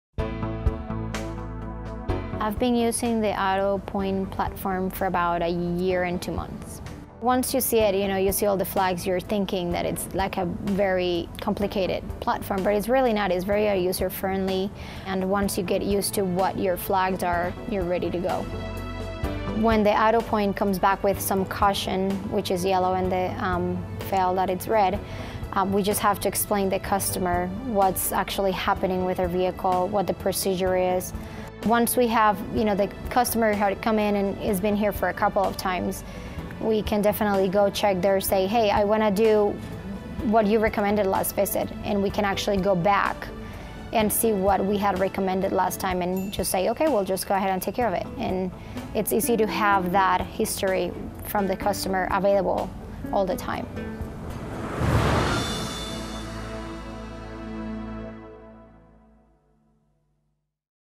AutoPoint Improves BMW Pleasant Grove's Customer Efficiencies
The AutoPoint Platform creates service department efficiencies that improve the customer experience, increase the service team's ability to get the job done and is very user friendly.  AutoPoint helps build customer trust with BMW Pleasant Grove.